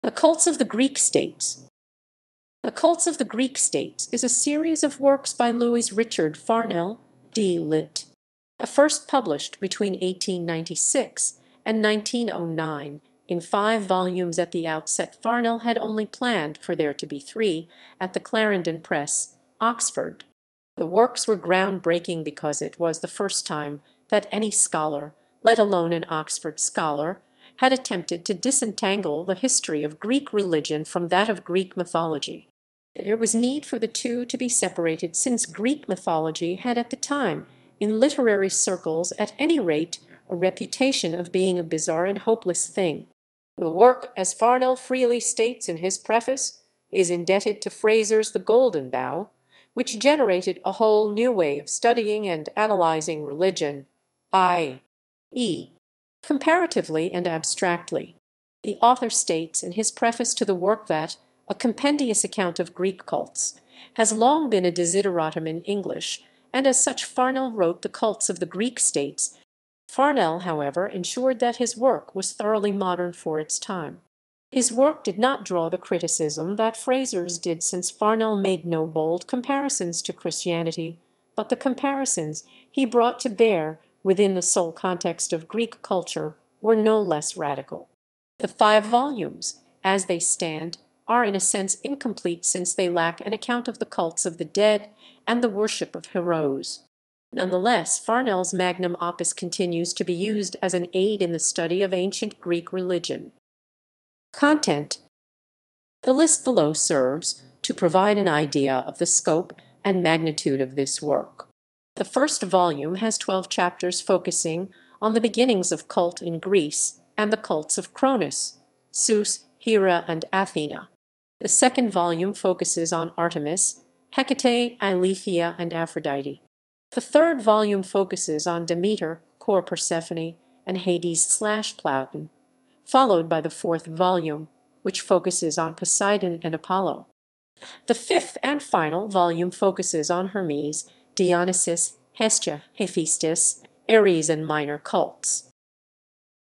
The Cults of the Greek States
The Cults of the Greek States is a series of works by Lewis Richard Farnell, D. Litt., first published between 1896 and 1909, in five volumes (at the outset Farnell had only planned for there to be three), at the Clarendon Press, Oxford. The works were groundbreaking because it was the first time that any scholar, let alone an Oxford scholar, had attempted to disentangle the history of Greek religion from that of Greek mythology. There was need for the two to be separated since Greek mythology had at the time, in literary circles at any rate, a reputation of being a "bizarre and hopeless thing".The work, as Farnell freely states in his preface, is indebted to Frazer's The Golden Bough, which generated a whole new way of studying and analysing religion, i. e. comparatively and abstractly. The author states in his preface to the work that, "a compendious account of Greek cults [...] has long been a desideratum in English," and as such Farnell wrote The Cults of the Greek States to sate that desire. Farnell, however, ensured that his work was thoroughly modern for its time. His work did not draw the criticism that Frazer's did since Farnell made no bold comparisons to Christianity but the comparisons he brought to bear within the sole context of Greek culture were no less radical.
The five volumes, as they stand, are in a sense incomplete since they lack "an account of the cults of the dead and the worship of heroes". Nonetheless, Farnell's magnum opus continues to be used as an aid in the study of ancient Greek religion.